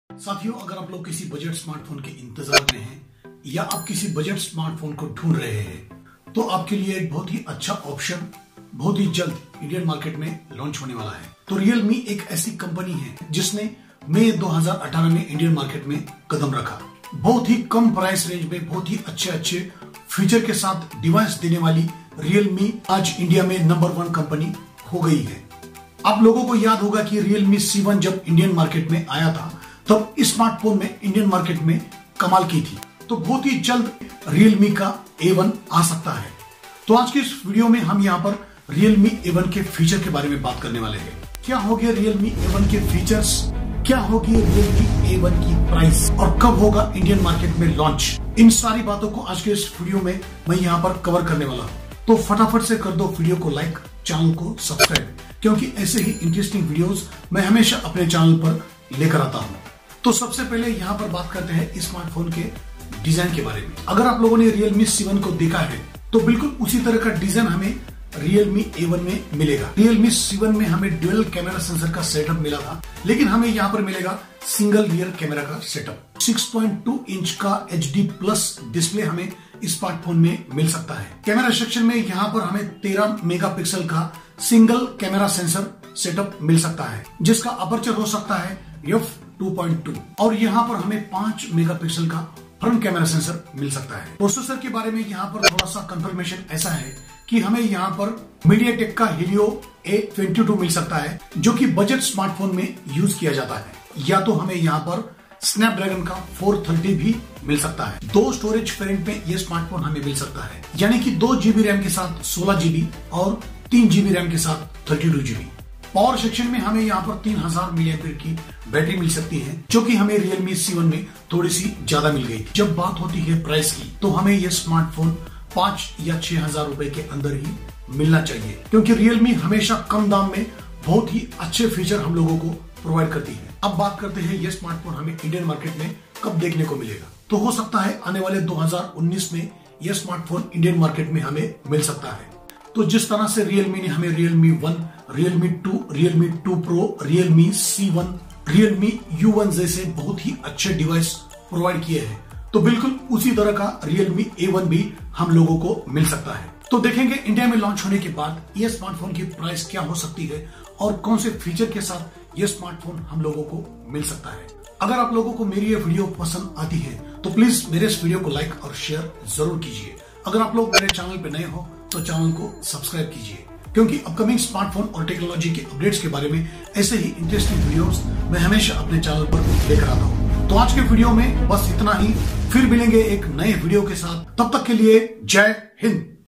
Realme A1 leaks, Review of specifications , Comming soon
Hi
Dosto aaj ke is video me Mai aapko
 Realme A1 ke leaks ke baare me batanewala hu... Umeed karta hu ye video aapko jarur pasand aayega

Background music full credit and provide by

NOTE:- ALL THE IMAGE/PICTURE SHOW IN VIDEO BELONG TO THE RESPECTED OWNERS AND NOT ME
I AM NOT OWNER OF ANY PICTURES SHOWED IN THE VIDEO.